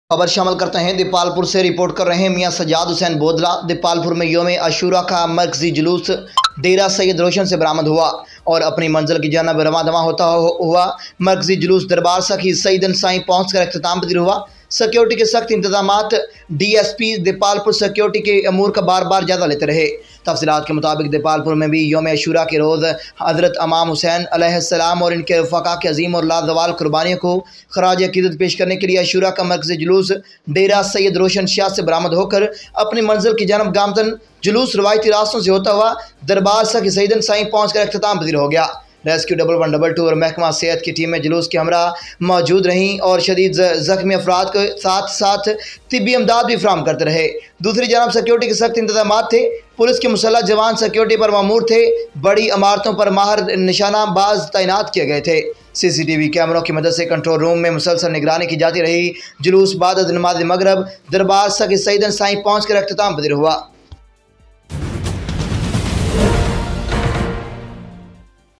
دیپالپور میں بھی یوم عاشورہ کے روز حضرت امام حسین علیہ سلام اور انکے رفقاء کی عظیم اور لازوال قربان
دیپالپور سے رپورٹ کر رہے ہیں میاں سجاد حسین بودلہ
دیپالپور میں یوم عاشورہ کا مرکزی جلوس ڈیرہ سید روشن سے برآمد ہو کر اپنی منزل کی جانب رواں دواں ہوتا ہو مرکزی جلوس دربار سخی سیدن سائیں پہنچ کر اختتام پذیر ہوا سکیورٹی کے سخت انتظامات ڈی ایس پی دیپالپور سکیورٹی امور کا بار بار جائزہ لیتےرہے

وی او۔۔
دیپالپور میں بھی یوم عاشورہ کے روز حضرت امام حسین علیہ سلام اور انکے رفقاء کی عظیم اور لازوال قربانیوں کو خراج عقیدت پیش کرنے کے لئے عاشورہ کا۔مرکزی جلوس ڈیرہ سید روشن شاہ سے برآمد ہو کر اپنی منزل۔کی جانب گامزن  جلوس روائیتی راستوں سے ہوتا ہوا دربار سخی سیدن سائیں پہنچ کر اختتام پذیر ہوگیا ریسکیو 1122اور محکمہ صحت کی ٹیمیں جلوس کے ہمراہ موجود رہیں اور شدید زخمی افراد کو ساتھ ساتھ طبی امداد فراہم کرتے رہے  دوسری جانب سکیورٹی کے بھی سخت انتظامات تھے پولیس کے مسلح جوان سکیورٹی پر مامور تھے بڑی عمارتوں۔پر ماہر نشانہ باز تعینات کیے گئے تھے سی۔سی ٹی وی کیمروں کی مدد سے کنٹرول روم۔میں مسلسل نگرانی کی جاتی رہی  جلوس بعد نماز مغرب دربار سخی سیدن سائیں پر پہنچ کر اختتام پذیر ہوا